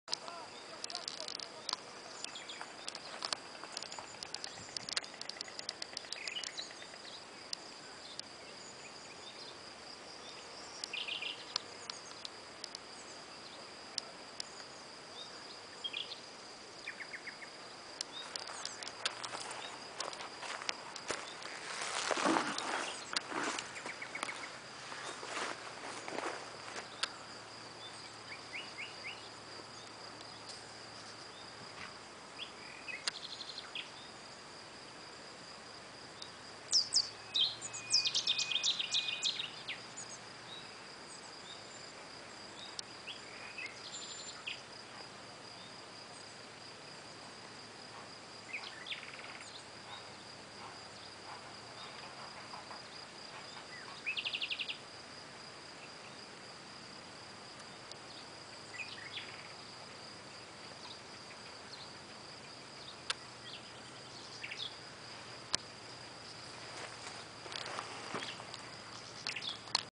Barragem da Marateca: Natureza ao vivo
Barragem da Marateca: Natureza ao vivo.Rota das cerejeiras em flor 25/33. Com partida de Castelo Novo, manhã na Serra da Gardunha e tarde em direcção à Barragem da Marateca. Em 2 de Maio de 2009.